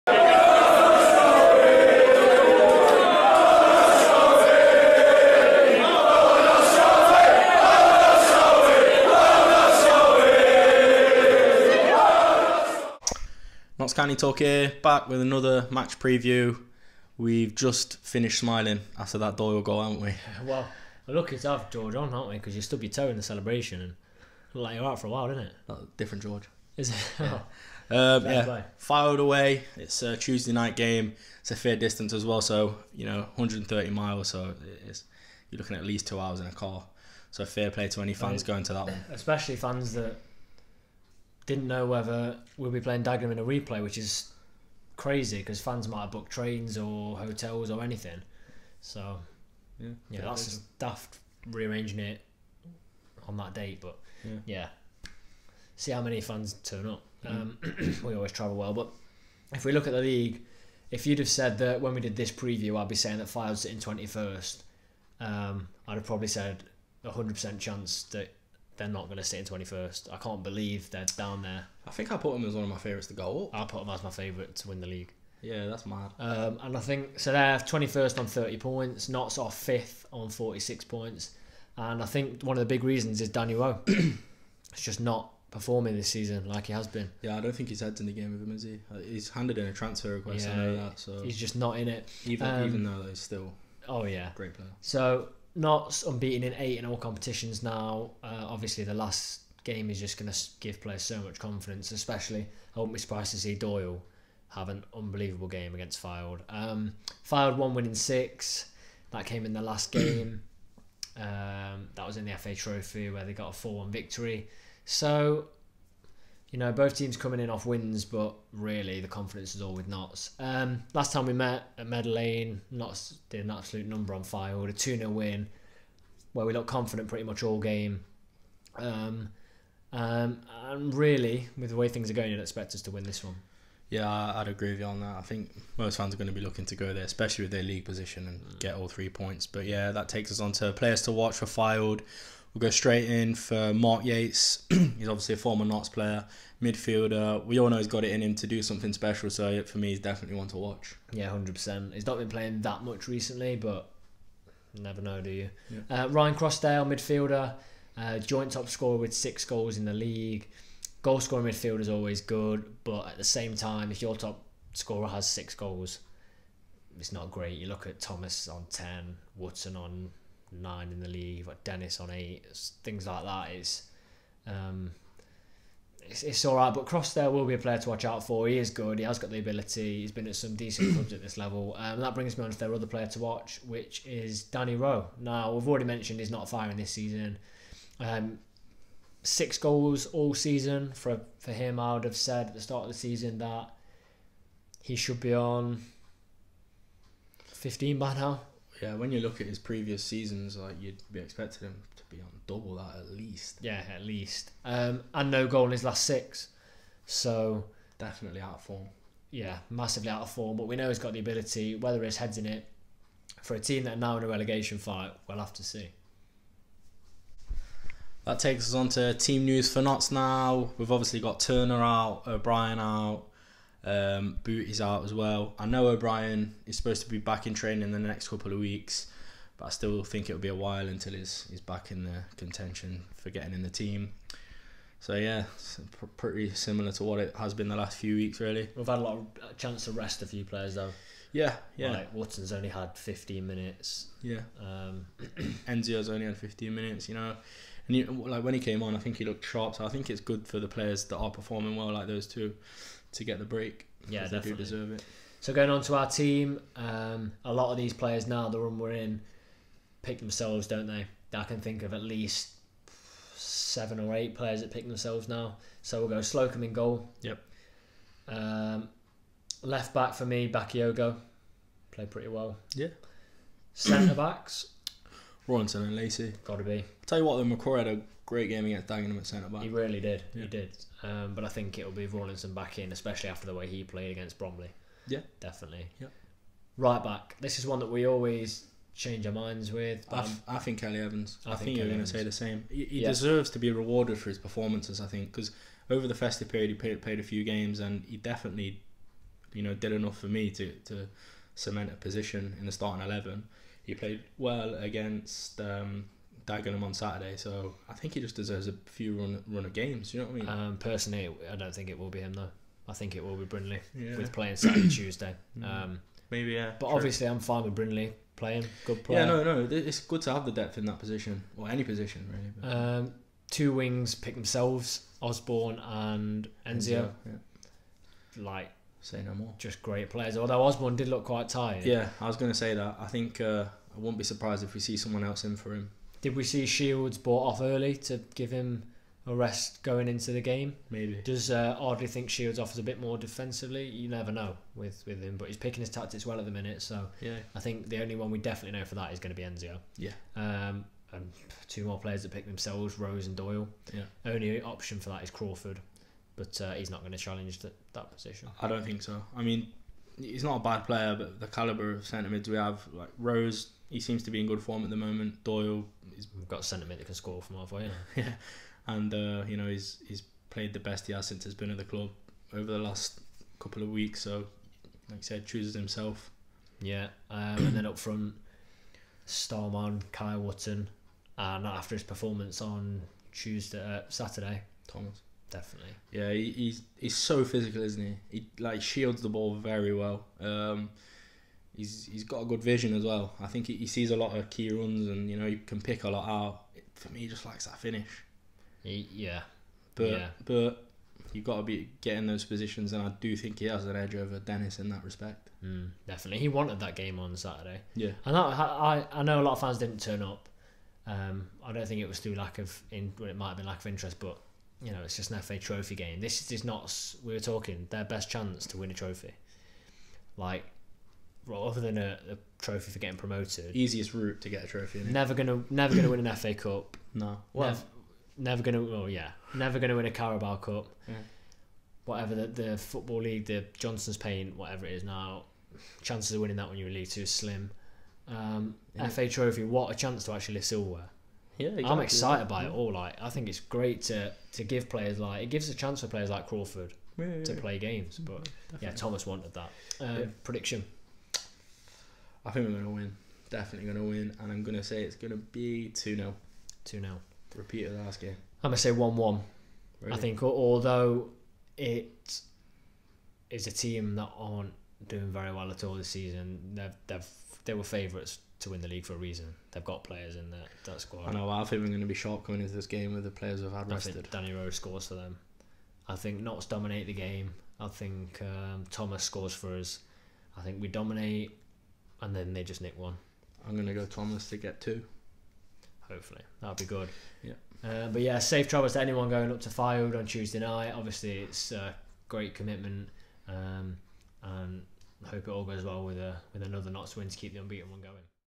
MATCH PREVIEW - AFC Fylde Vs Notts County
Quick round up of all you need to know before we travel to AFC Fylde Tuesday night.